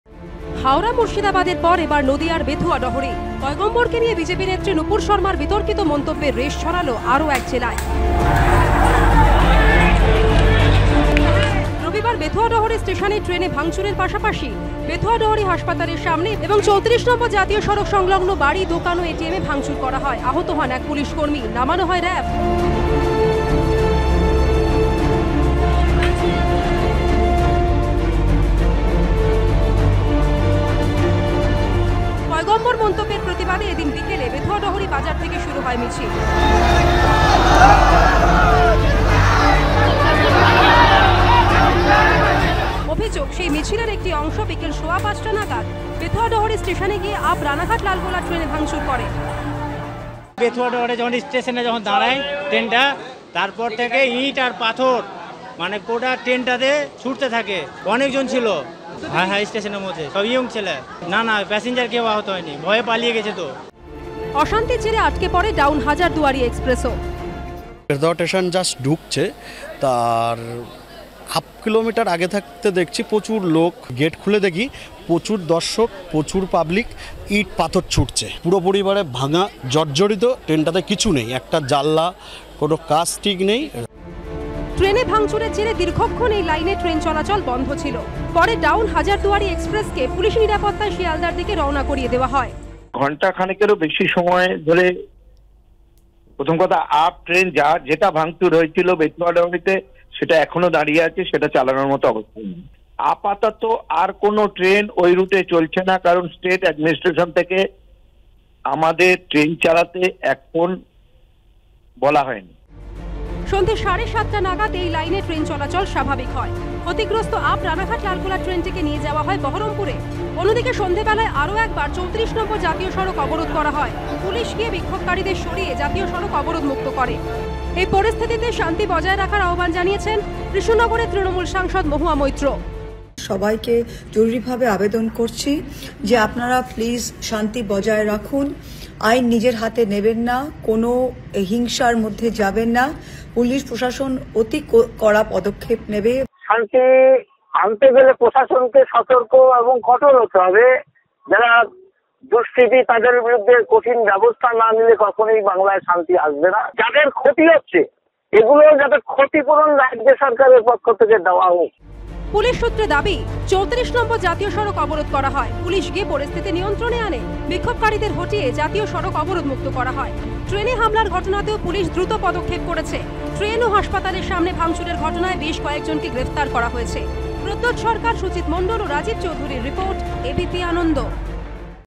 Bethuadahari Train Attack : বেথুয়াডহরি স্টেশনে ট্রেনে ভাঙচুর, শান্তির আহ্বান মহুয়া মৈত্রর : ABP Ana
পয়গম্বরকে নিয়ে বিজেপি নেত্রী নূপুর শর্মার মন্তব্যের আঁচ নদিয়াতেও। বেথুয়াডহরি স্টেশনে ট্রেনে ভাঙচুর। বেথুয়াডহরি হাসপাতালের সামনে ও ৩৪ নম্বর জাতীয় সড়কেও দোকান ও বাড়িতে ভাঙচুর করা হয়। পরে, পুলিশ পরিস্থিতি নিয়ন্ত্রণে আনে। তাণ্ডবের প্রতিবাদে ৭২ ঘণ্টা ব্যবসা বন্ধের ডাক দিয়েছে বেথুয়াডহরি ব্যবসায়ী সমিতি।